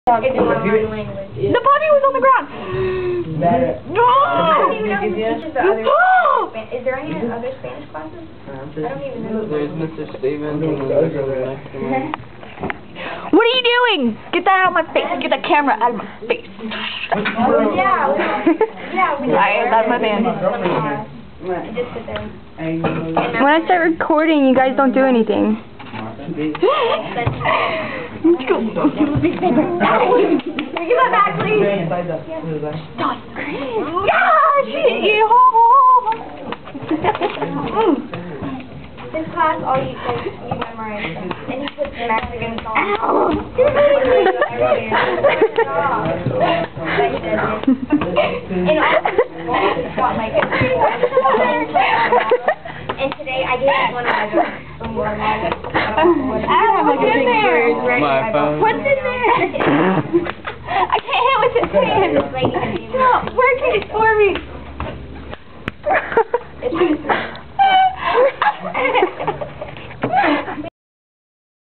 Paper ball
Kayla, Chris, and Monica being crazy in that one class. Named in order of crazyness.